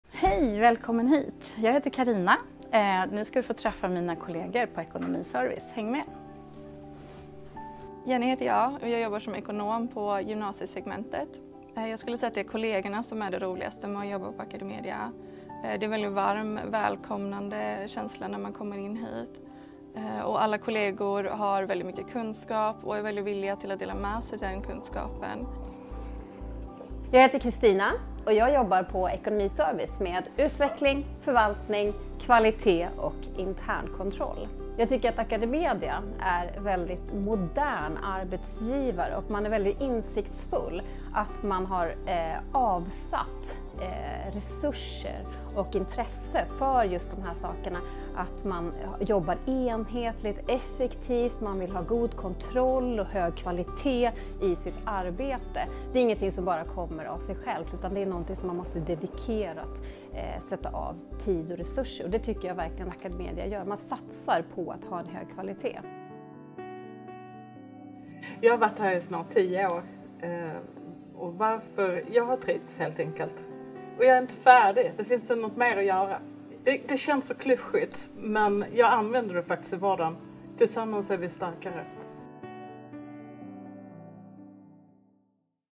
Säg hej till AcadeMedias Ekonomiservice!
Så kul att du är nyfiken på oss som jobbar på AcadeMedias Ekonomiservice. Här får du en inblick i vilka vi är och vad vi gör på dagarna.